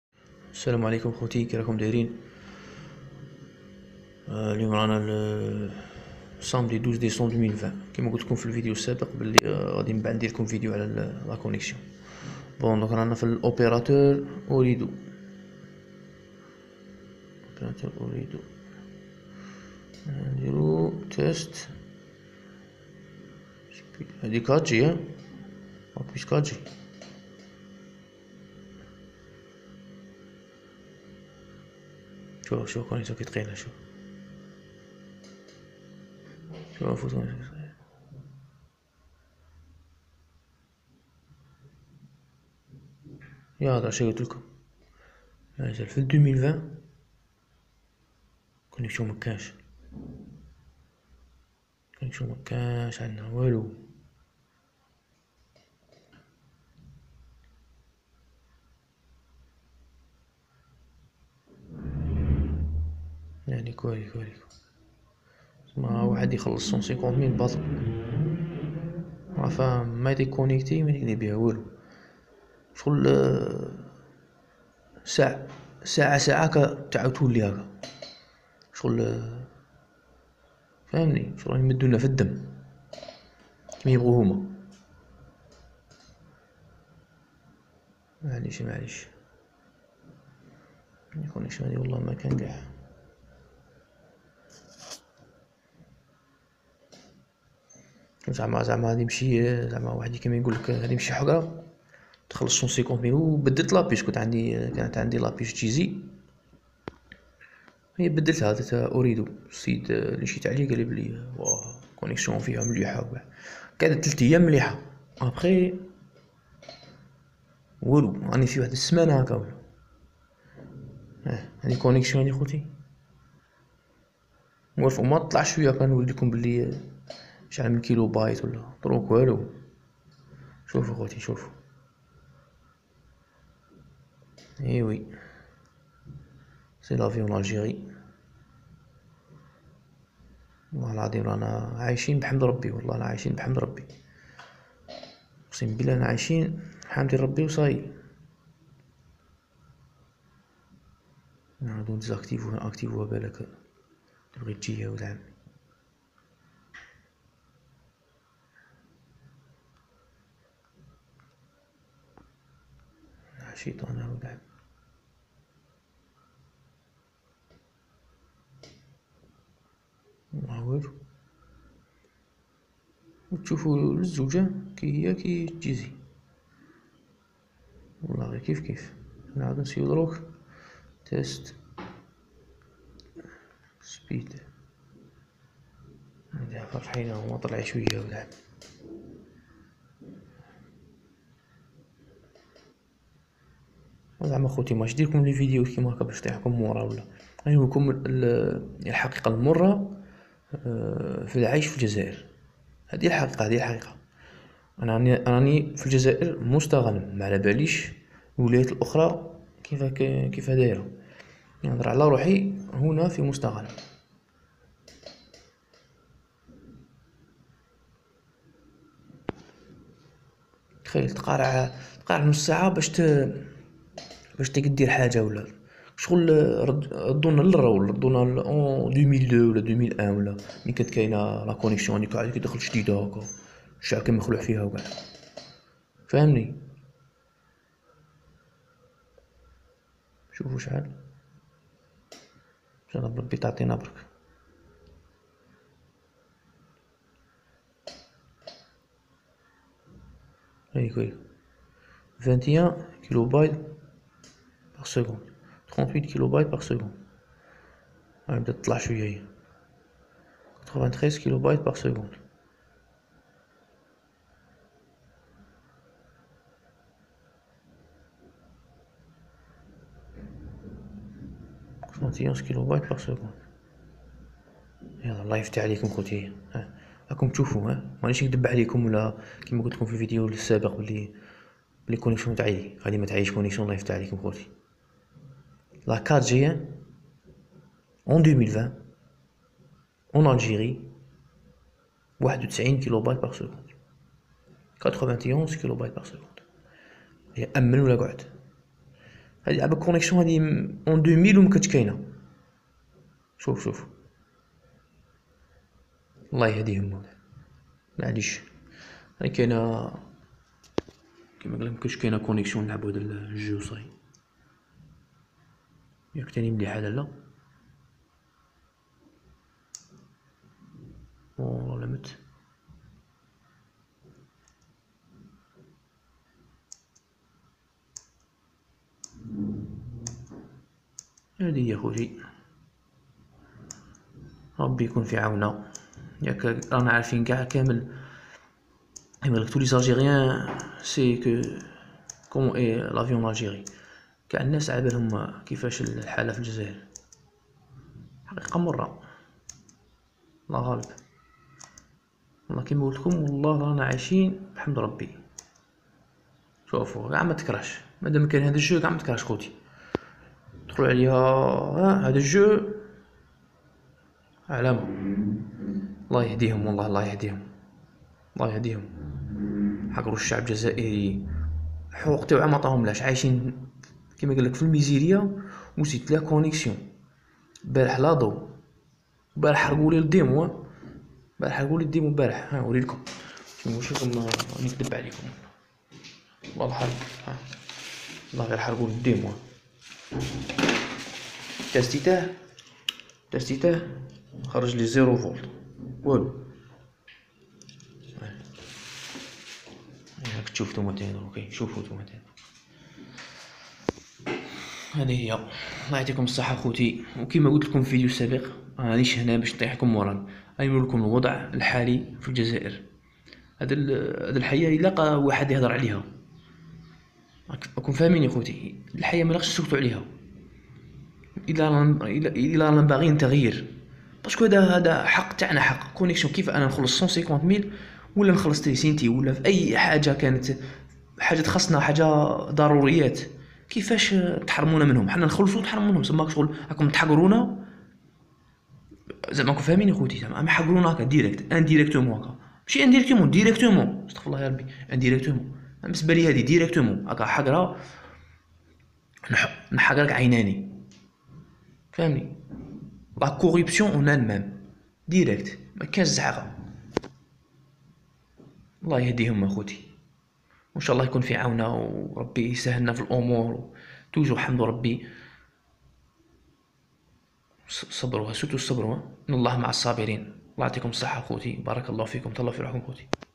La vie en Algérie "3" |الحياة في الجزائر"3" | أسرع اتصال إنترنت في العالم في الجزائر 2020
La connexion la plus rapide au monde hhhhhhh rabhaa tkhaylo khouti 4g en Algerie 2020 la rapiditè taha 91 kbps 🤔 🤔 🤔 🤔 🤔 🤔 🤔 no comments

لا تنسو الإشتراك والإعجاب بالقناة وتفعيل زر الجرص ليصلكم كل جديد حول الحياة في الجزائر هههههههههههه